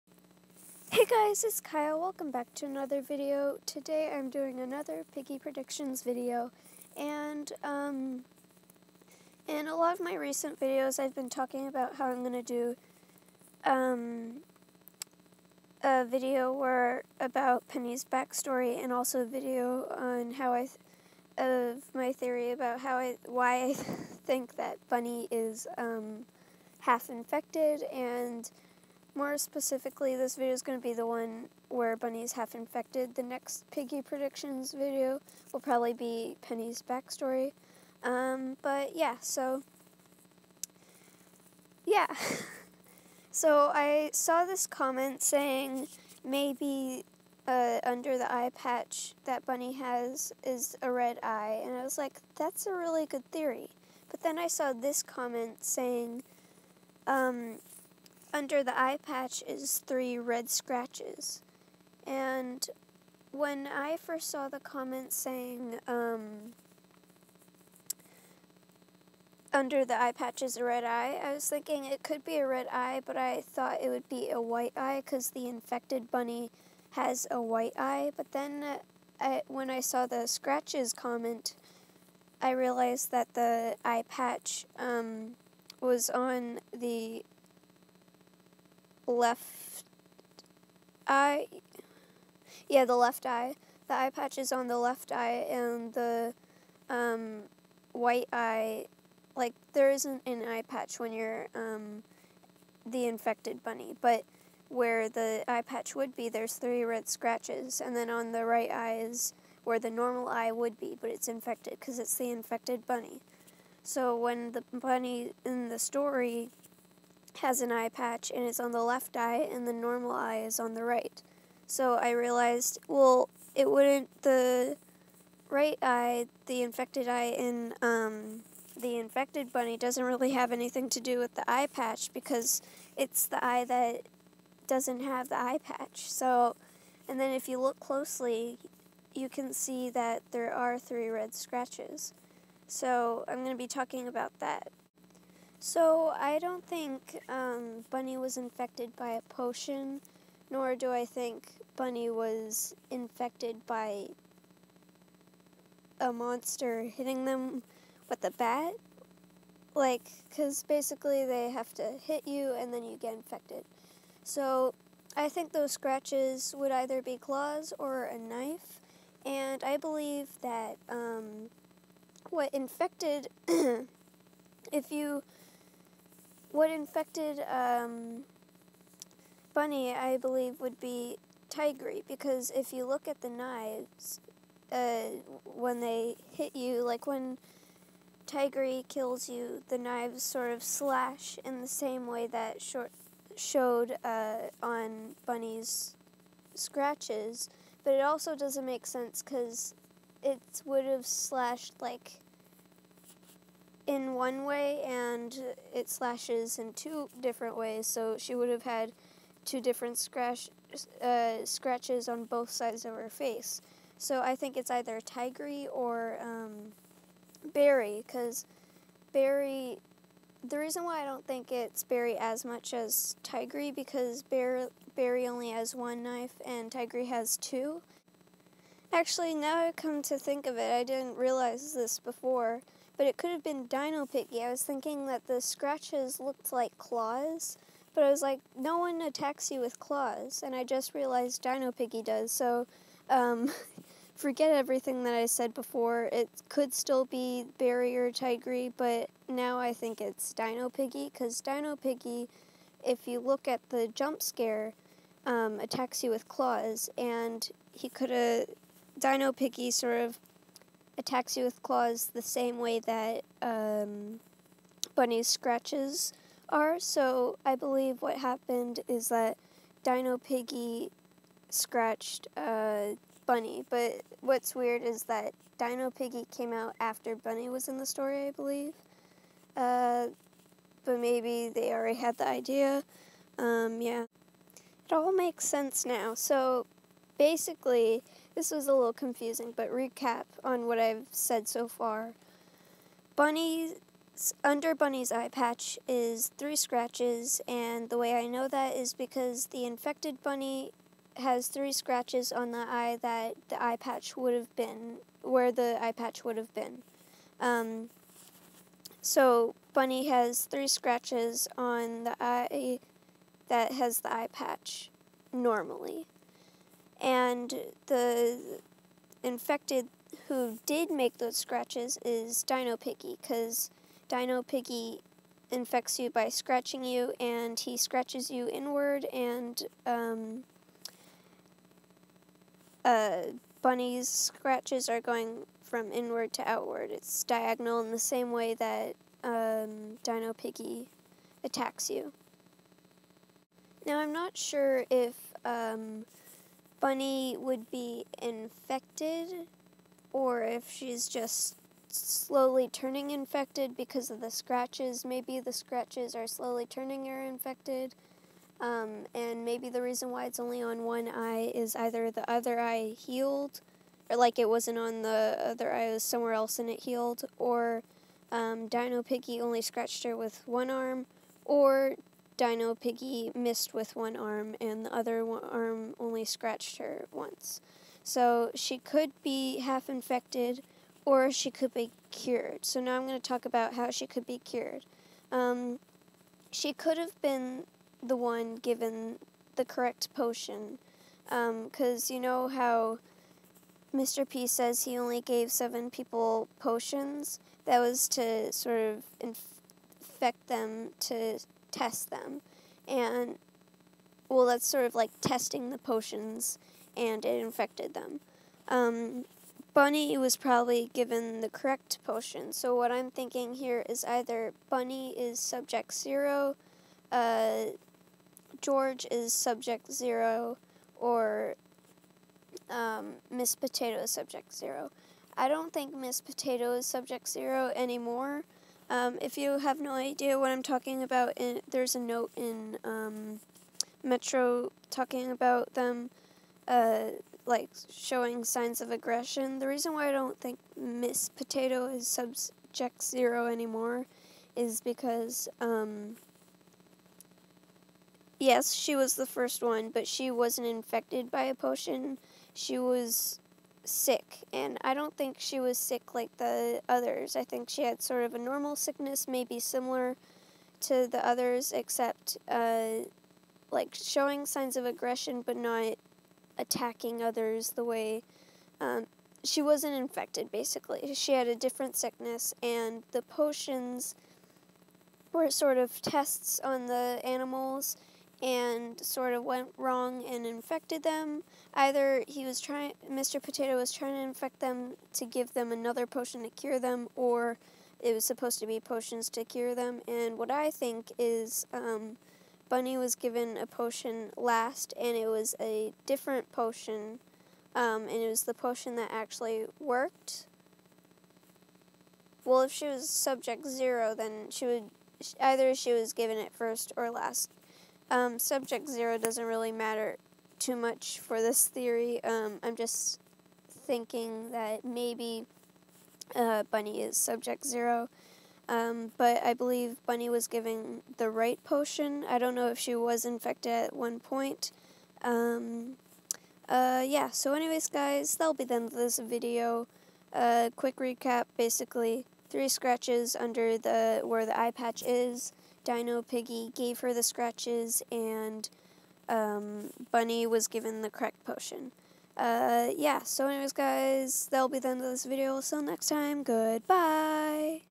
What’s Underneath Bunny’s Eyepatch? || Roblox Piggy Predictions
In this video I explain my theory on what is under Bunny’s eyepatch and the truth about her.